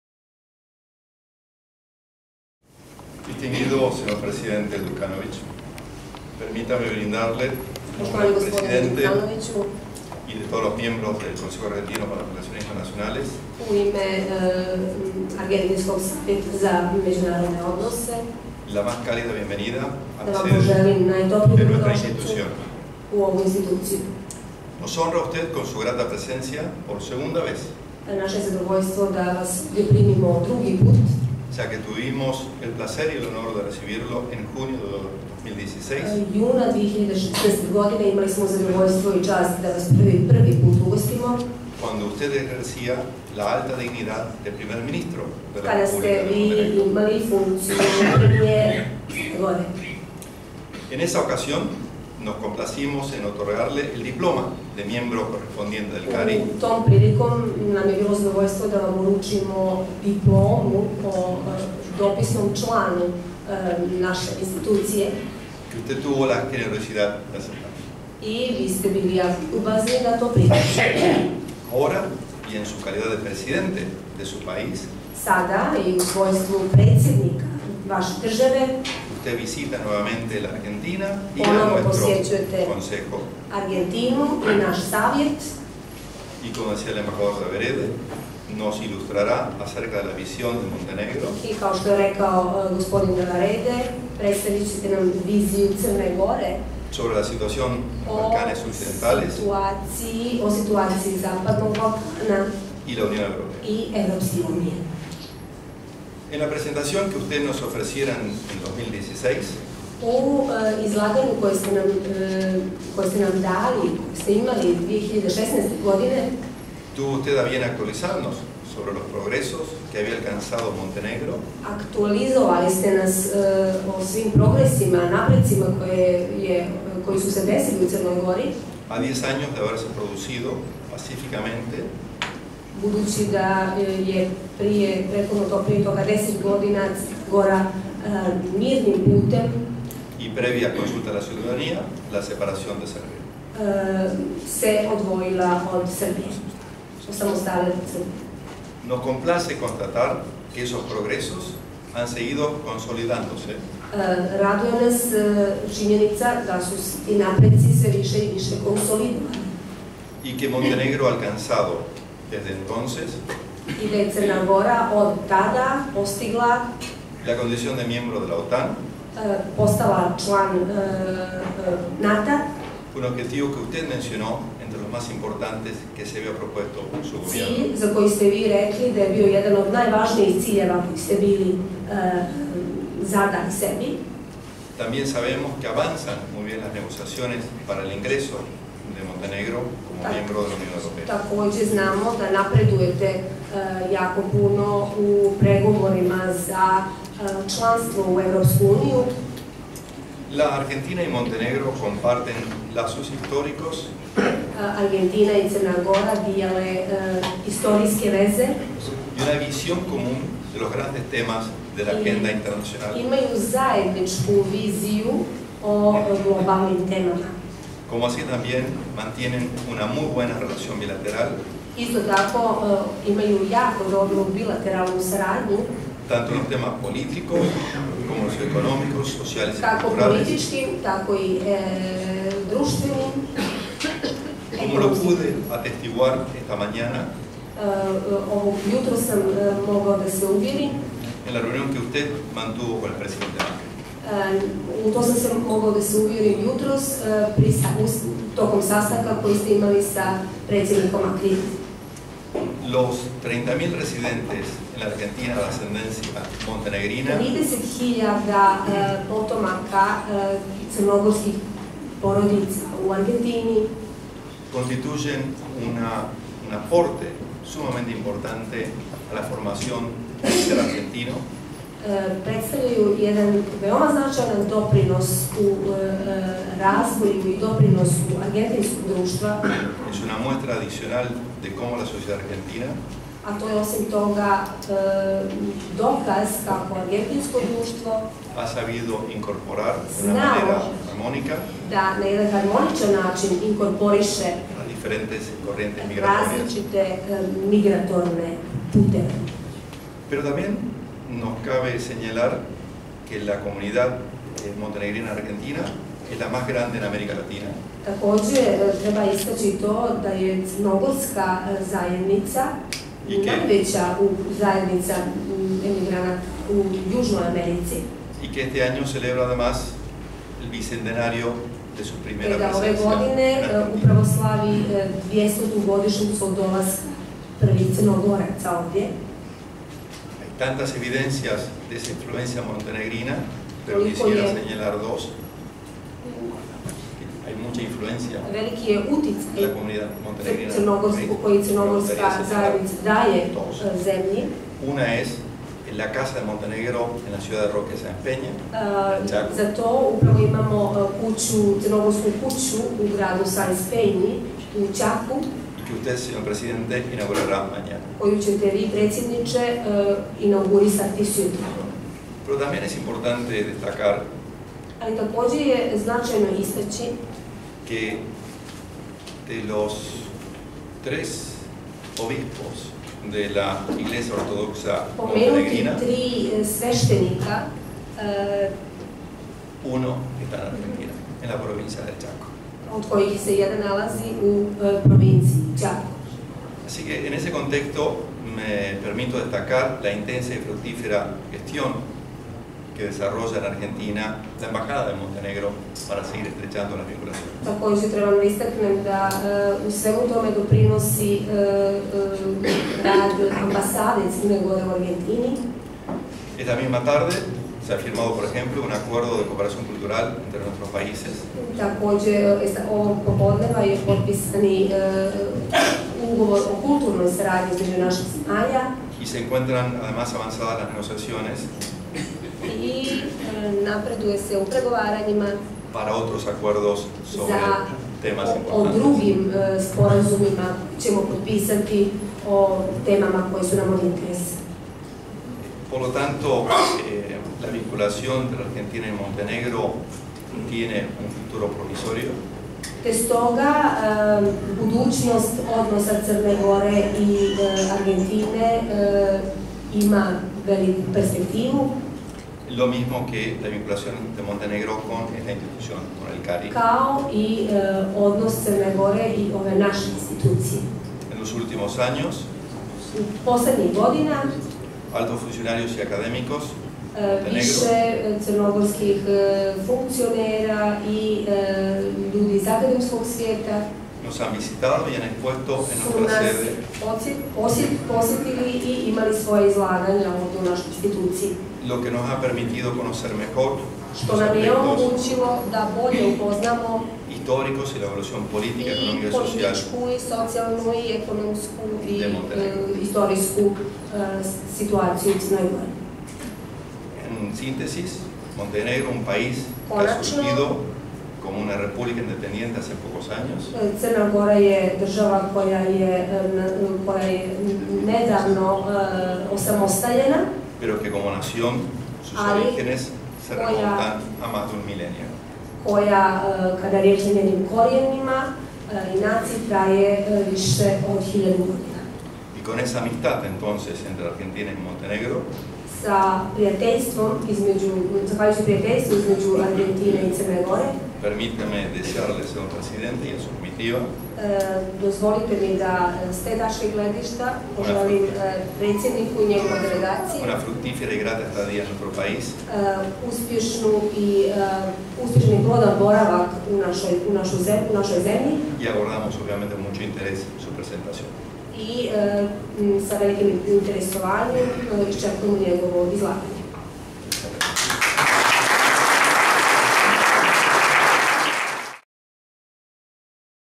2018-12-10 Balcanes Occidentales y UE después del Brexit. La visión de Montenegro-ZLAUVINEN-PARTE 1
Lunes 10 de diciembre

Los Balcanes Occidentales y la Unión Europea después del Brexit. La visión desde Montenegro

Sesión académica a cargo de S. E. D. Milo Đukanović, Presidente de Montenegro, organizada por el Comité de Asuntos Europeos

Milo Đukanović / Presidente de Montenegro. Se graduó de la Facultad de Economía de Podgorica, Universidad de Montenegro. Fue elegido, por primera vez, Presidente de Gobierno el 15 de febrero de 1991. Cumplió con esa función durante siete mandatos entre 1991 y 2016. En 1998 fue electo Presidente del Partido Demócrata de los Socialistas. Fue, asimismo, el líder del Bloque para la Independencia de Montenegro. Fue diputado en el Parlamento de Montenegro durante varios mandatos. El 20 de mayo de 2018 asumió funciones como Presidente.